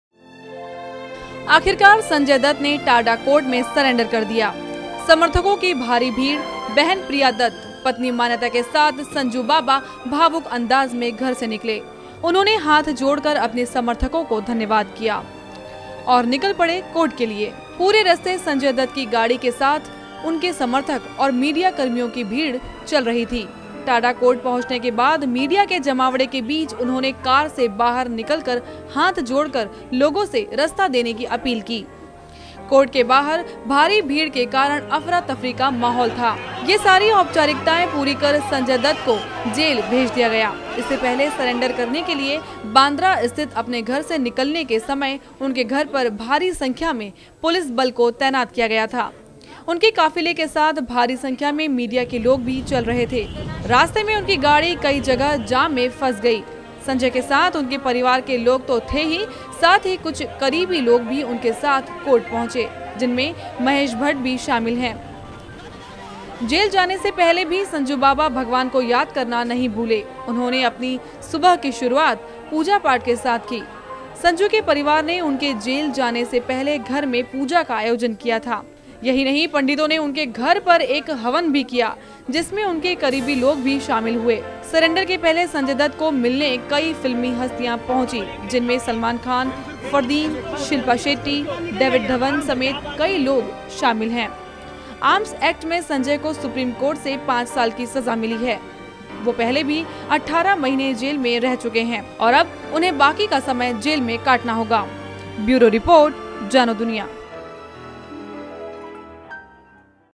Sanjay Dutt aka 'Munnabhai' goes to jail - JanoDuniya
It is now endgame for Sanjay Dutt in the 20-year-long saga that saw him charged with illegal arms possession in the 1993 Bombay blasts. The actor surrendered in a TADA court in Mumbai and spent the night in the Arthur Road jail. He will be moved to Pune's Yerawada Jail to begin the 42 months left of the five-year jail term he was handed by the Supreme Court. As he left the Imperial Heights building in Bandra, he was accompanied by wife Manyata, film director Mahesh Bhatt and some family members. Wearing a while kurta and blue jeans, he was seen holding Manyata's hand.